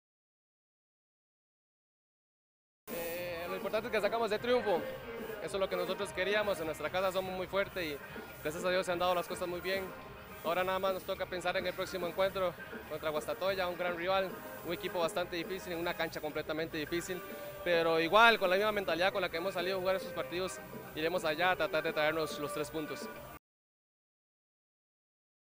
Adrian de Lemos | Antigua GFC 3-1 Xelajú MC
De Lemos se perfila como el menos vencido tras cuatro jornadas disputadas del Clausura 2018